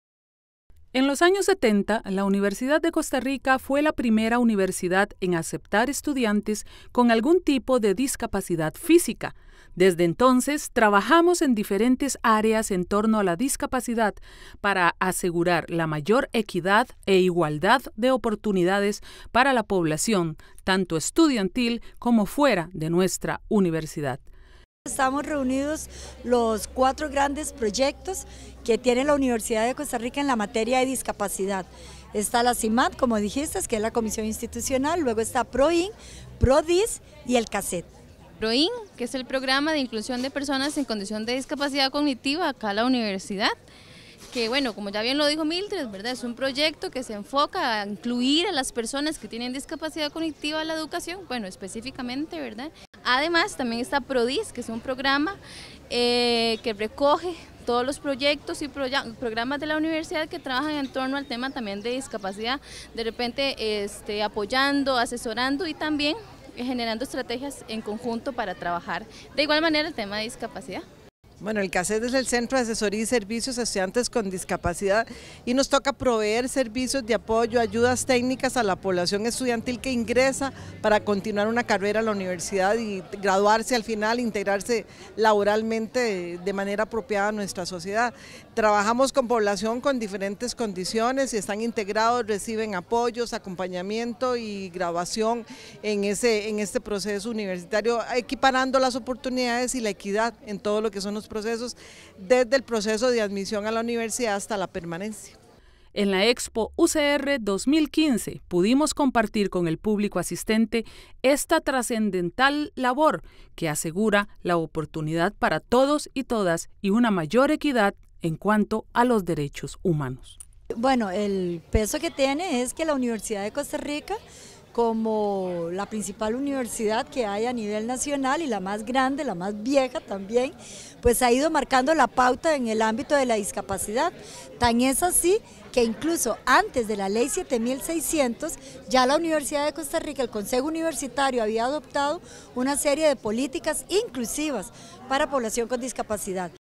Expo UCR CIMAD
Edición especial desde la EXPO UCR 2015. Evento con el cual la Universidad de Costa Rica dio inicio a las actividades de celebración de su 75 aniversario.